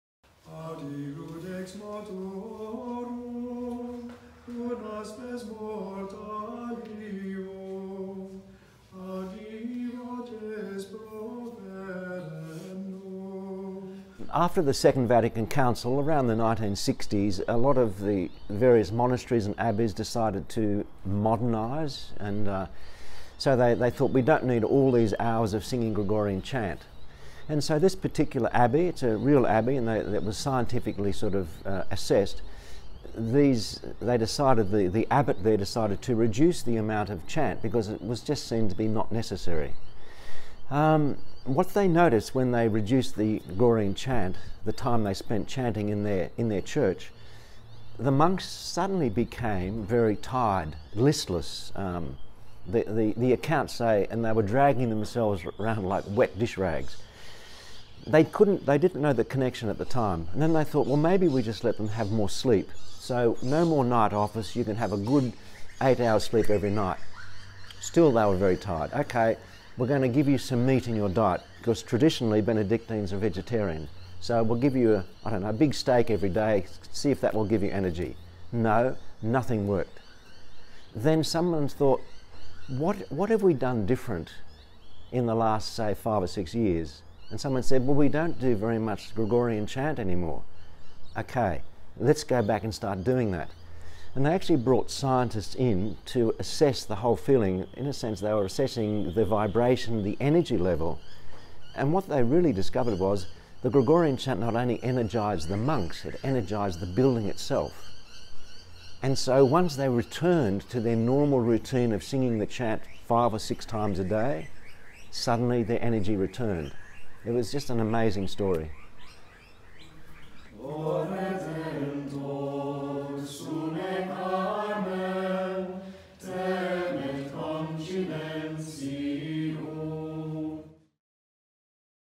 Gregorian chant with Tony Vaughan of Schola Cantorum - Part 2 of 3
Gregorian Chant and its origins and influence on music we know today. Filmed at the Abbey Museum, October 2019.